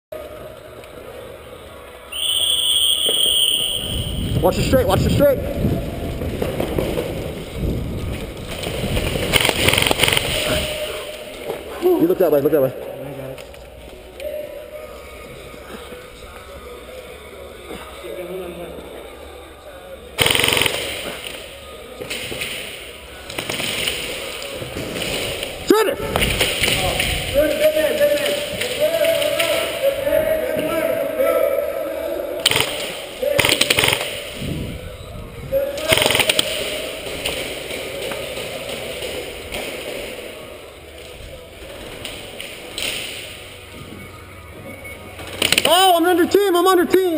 Airsoft GI Uncut - Daniel and Brian @ Tac City South 001
Daniel and his friend Brian from the Marines go to Tac City South for some airsoft fun.  Everything is going quite well until some unexpected friendly fire.  Check your targets first!  Look forward to more game play footage in the near future.